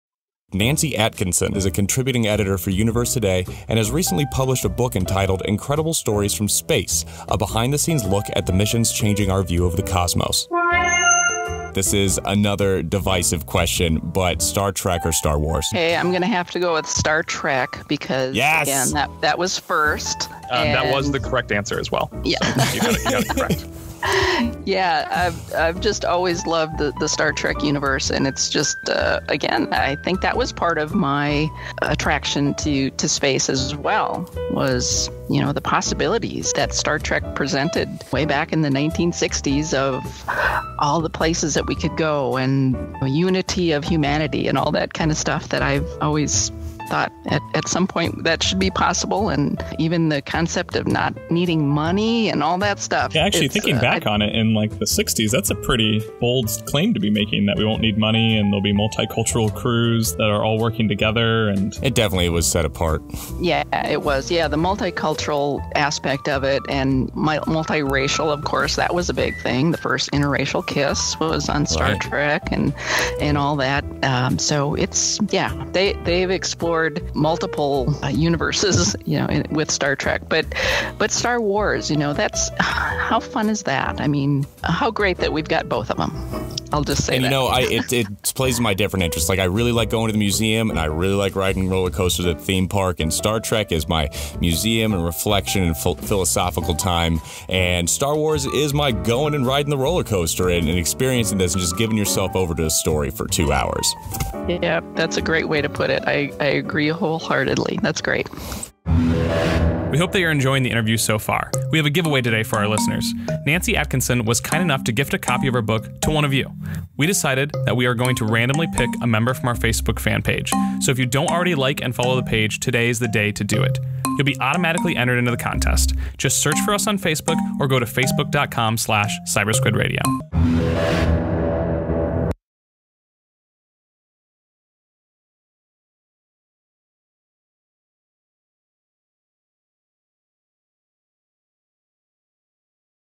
Star Trek vs. Star Wars Nancy Atkinson Chimes In
Join Jeff and Logan as they interview Nancy Atkinson - a science journalist and contributing editor to Universe Today. Nancy Atkinson interviewed over 30 NASA engineers and scientists to get the in-depth scoop on some of the most famous space missions of our lifetime. Her experience in the field has her brimming with stories. She shares with us a few of her favorite tales, and Jeff tries to pitch a bad book idea for her next project. If you like NASA, you’ll love this episode and her book, Incredible Stories from Space.